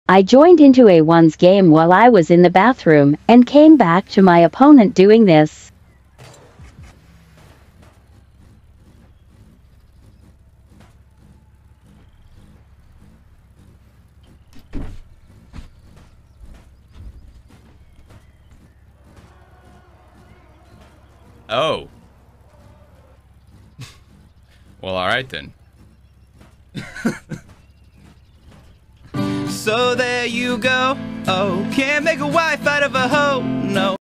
Joined a 1s game while I was in the bathroom and came back to this...
Ayo Santa is kinda sus...
►I'm LIVE every week on Twitch Mon-Fri!